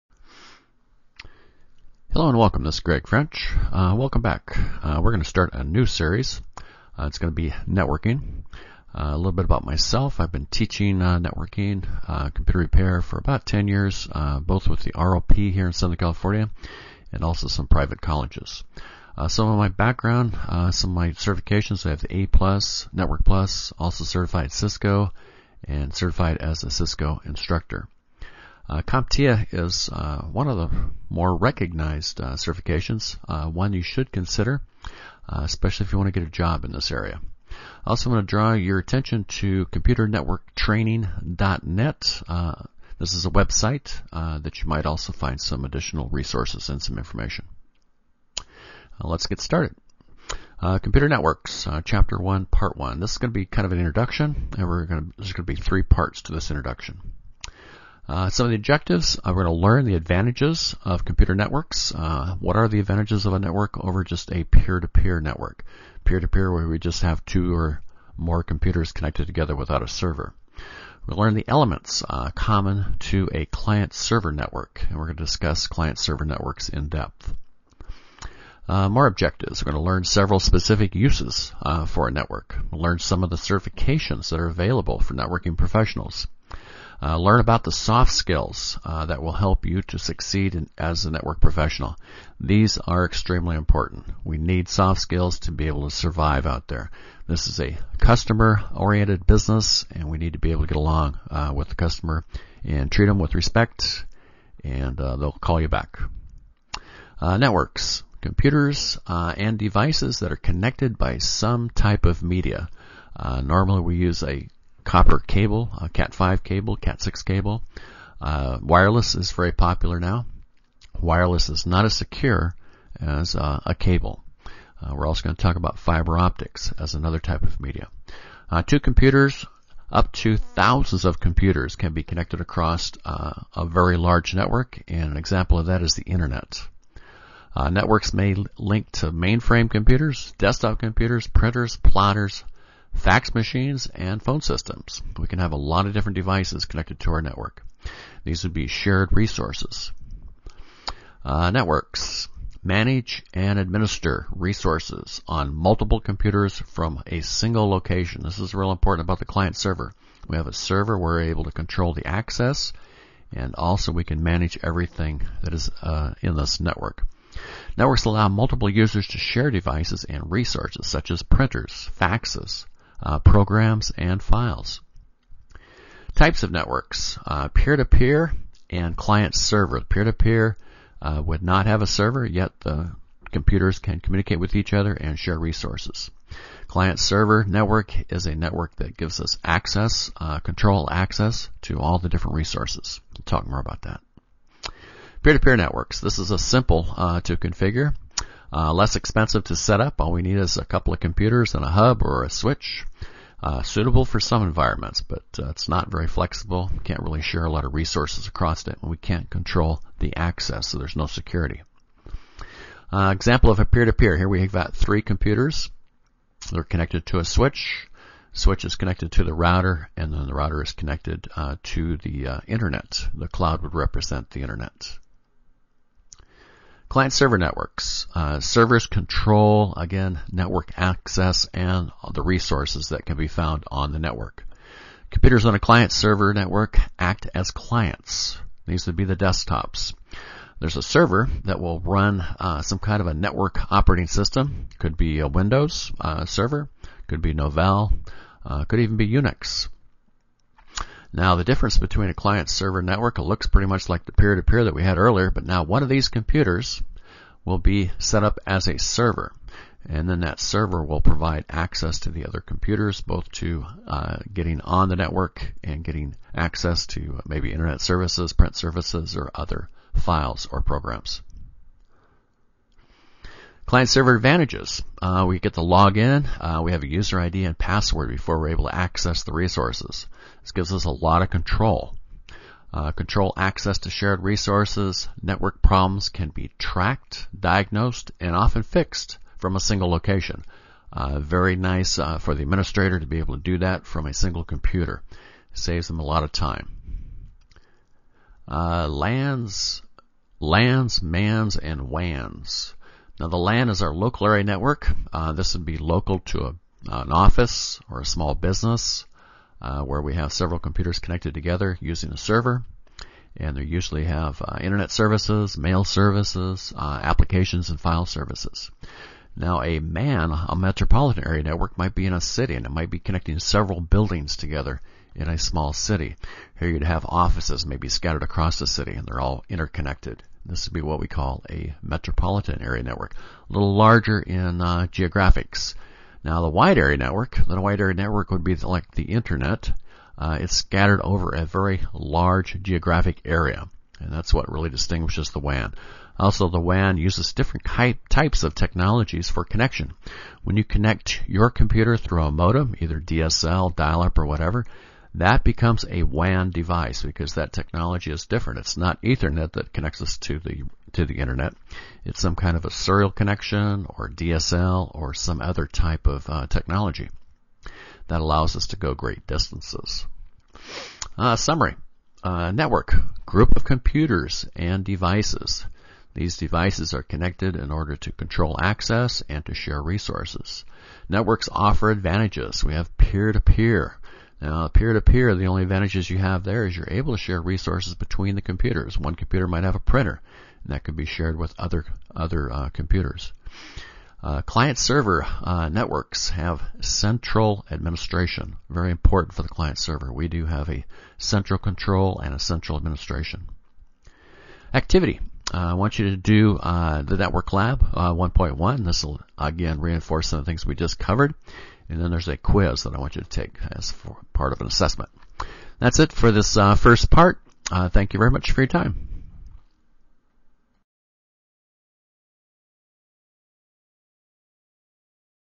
Computer Network Training Course 1.1- Topologies
Computer Networking Training is an introduction to computer networking and the first part of a new training series.  This introduction discusses basic network types, topologies, protocol, LAN, MAN, and WAN networks.